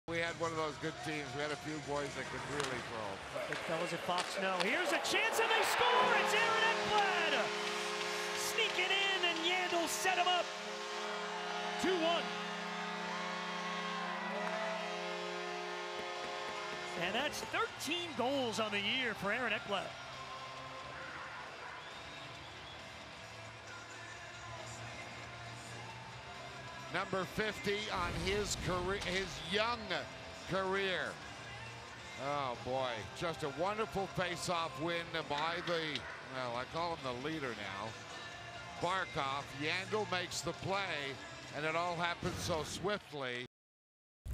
Aaron Ekblad Goal vs BUF 03-02-2018
Aaron Ekblad 13 goal of 2017-18 nhl season. even goal assisted by Keith Yandle and Evgeni Dadonov